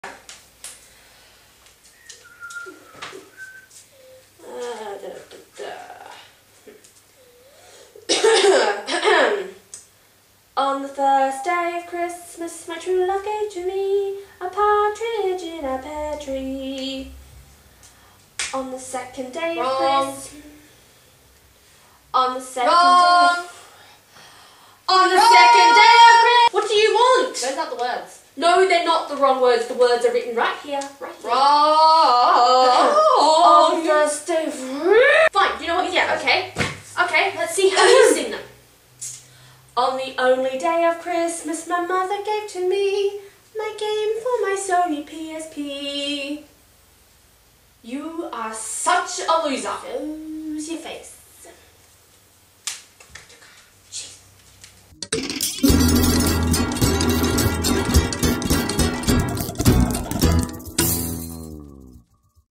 The Christmas Lyrics (Vlogcember Day 25)
Yes I know the editing is crap, but I liked the idea so I put it up. :) Merry Christmas everyone! :D